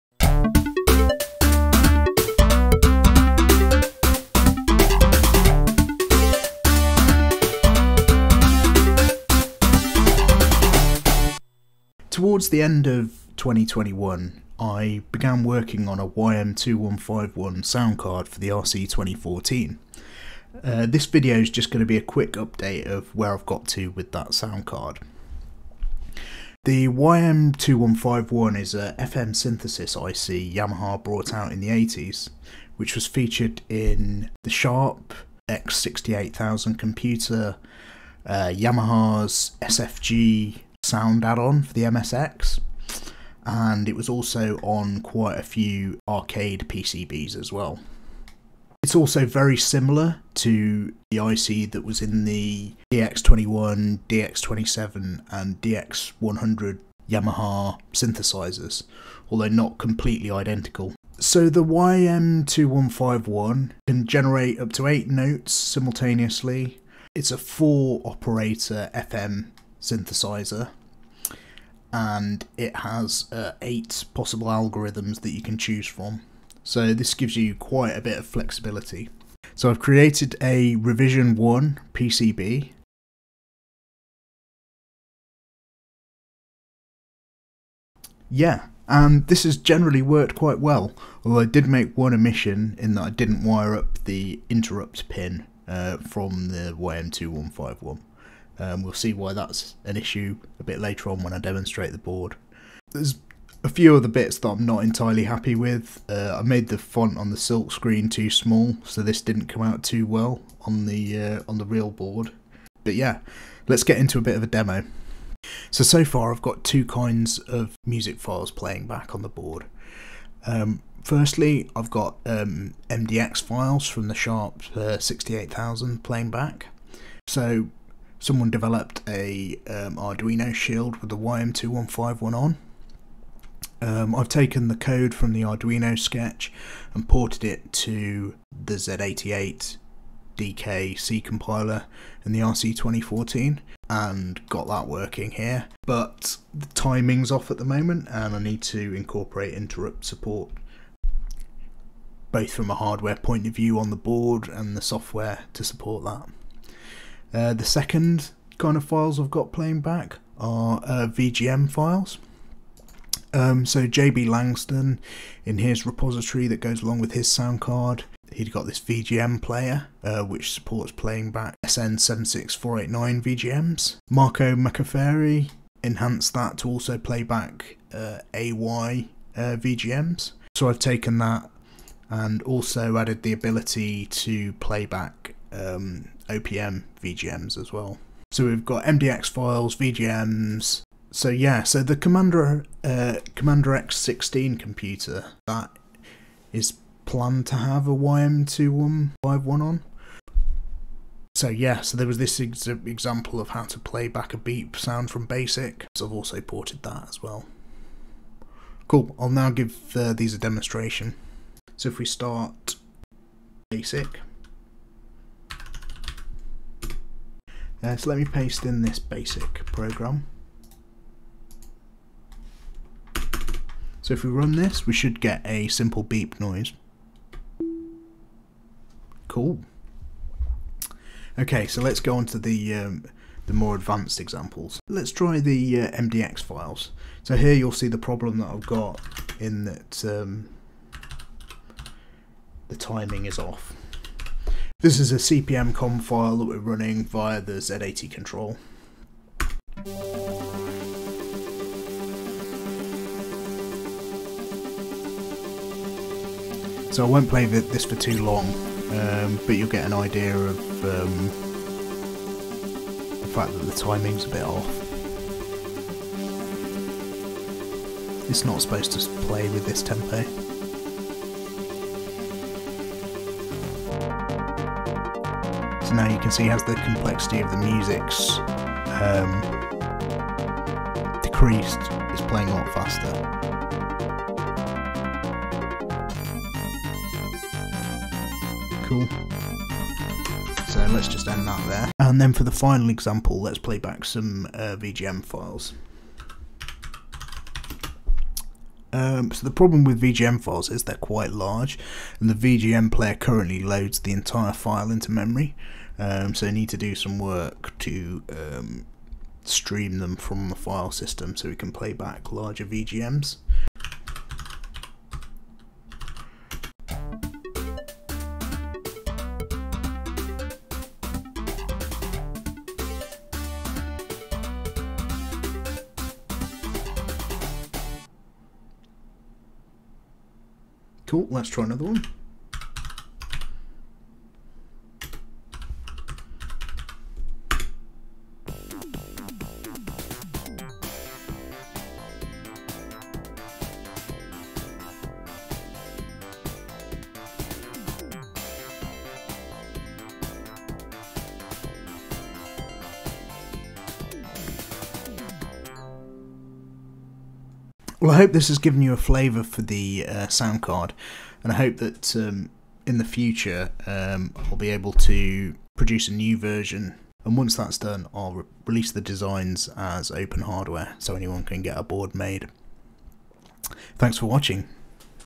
RC2014 OPM / YM2151 Sound Card Status Report
A status report on my OPM sound card, a follow on from my previous AY/YM2149 sound card.

Quick and dirty video. Re-upload to fix audio quality! (I'd overdone it with the normalisation)